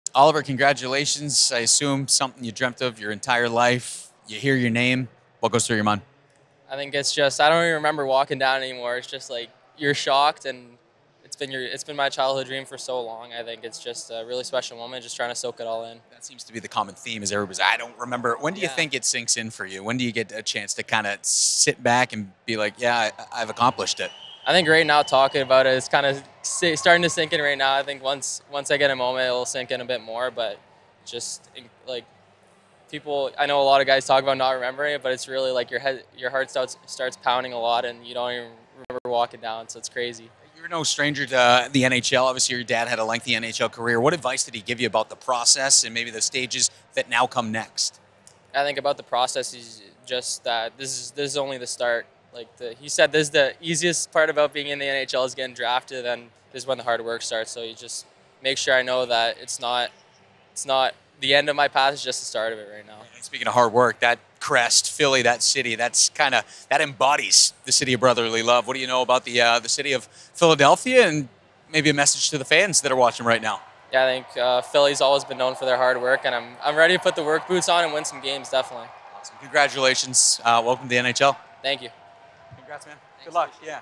Oliver Bonk (Philadelphia Flyers) NHL Network Interview - 2023
After being selected 22nd overall by the Philadelphia Flyers London Knights defenceman Oliver Bonk speaks with Jamison Coyle of the NHL Network.